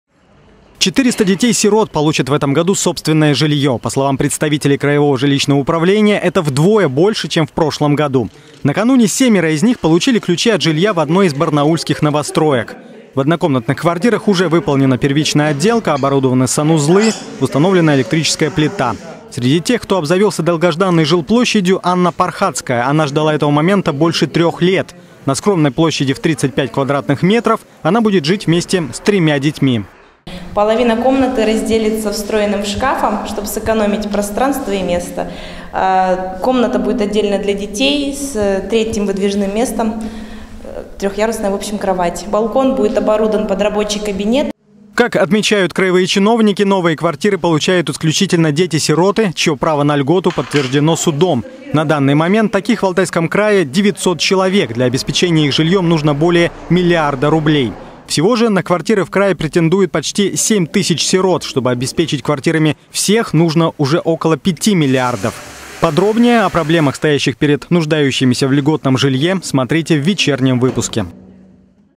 400 детей-сирот получат в этом году собственное жилье
По словам представителей краевого жилищного управления, это вдвое больше, чем в прошлом году. Накануне семеро из них получили ключи от жилья в одной из барнаульских новостроек.

В однокомнатных квартирах уже выполнена первичная отделка, оборудованы санузлы, установлена электрическая плита. Среди тех, кто обзавелся долгожданной жилплощадью, Анна Пархатская. Она ждала этого момента больше трех лет. На скромной площади в 35 квадратных метра она будет жить вместе с тремя детьми.